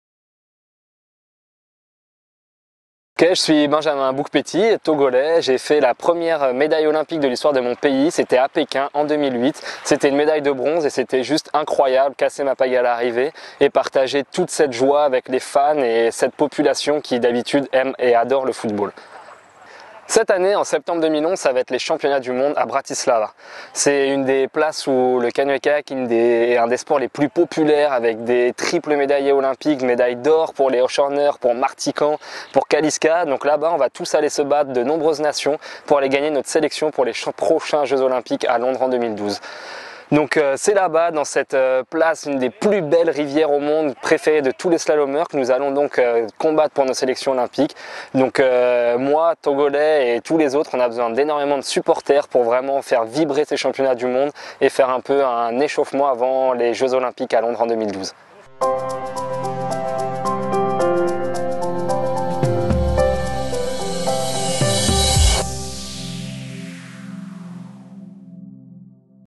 Benjamin Boukpeti invites you to Bratislava 2011!
The 2011 ICF Canoe Slalom World Championships will take place in Bratislava, Slovakia, from September 6th to September 11th! What does Benjamin Boukpeti think about it?